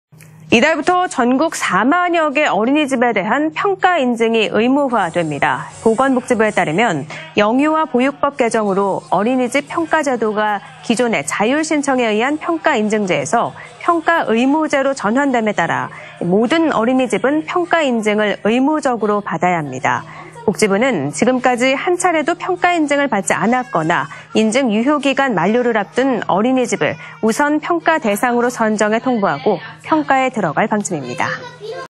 6월부터 어린이집 평가인증 의무화 / 안동MBC
2019/06/08 10:00:30    작성자 : 조동진

이달부터 전국 4만여 개 어린이집에 대한
평가인증이 의무화됩니다.
◀ＥＮＤ▶

보건복지부에 따르면 영유아보육법 개정으로
어린이집 평가제도가 기존의 자율 신청에 의한
평가인증제에서 평가의무제로 전환됨에 따라
모든 어린이집은 평가인증을 의무적으로
받아야 합니다.

복지부는 지금까지 한 차례도 평가인증을 받지
않았거나 인증 유효기간 만료를 앞둔
어린이집을 우선 평가대상으로 선정해 통보하고
평가에 들어갈 방침입니다.